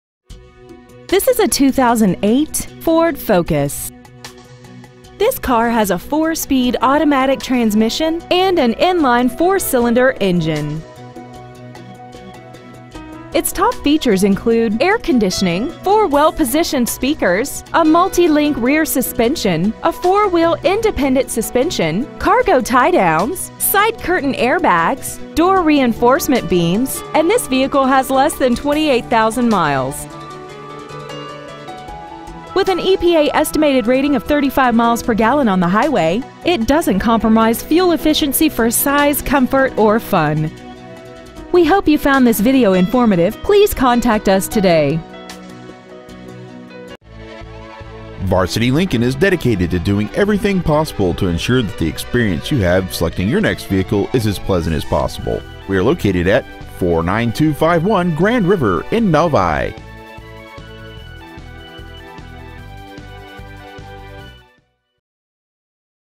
2008 FORD FOCUS Novi MI
We have been honored to serve the Novi MI area , we promise that your experience at our dealership will exceed your expectations !

 Year : 2008
 Make : FORD
 Model : FOCUS
 Engine : 2.0L I4
 Trans . : AUTO 4SPD

 Miles : 27,520

 Stock : 33022